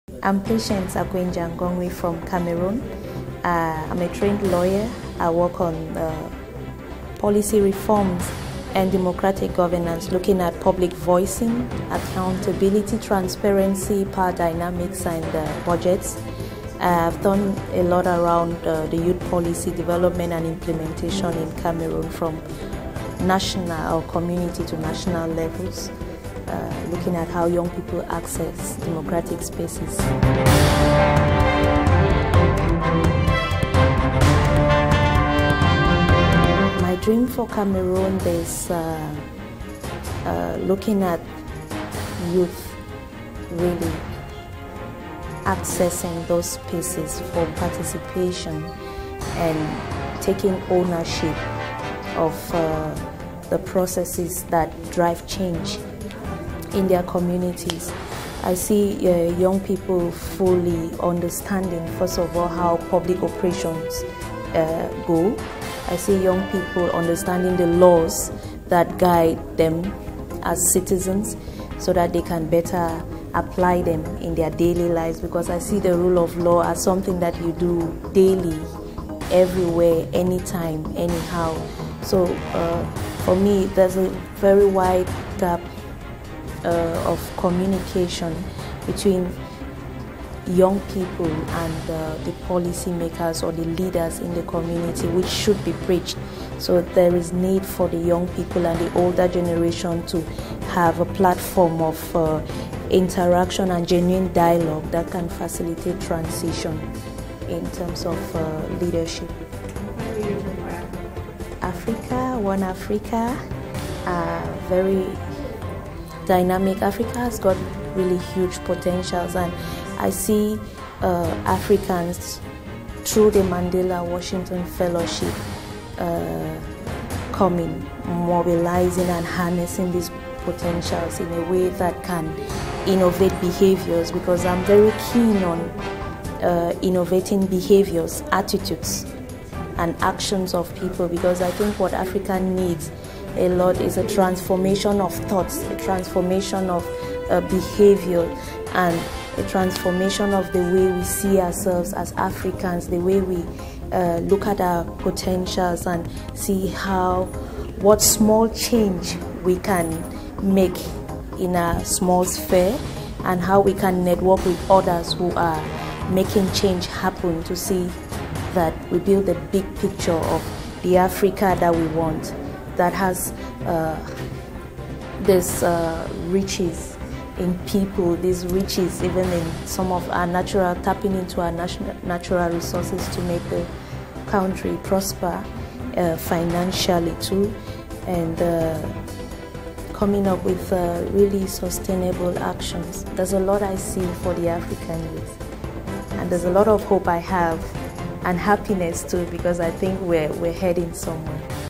They Have a Dream - YALI Fellow Cameroon
YALI Africa Fellows talk about their dream for their country and Africa.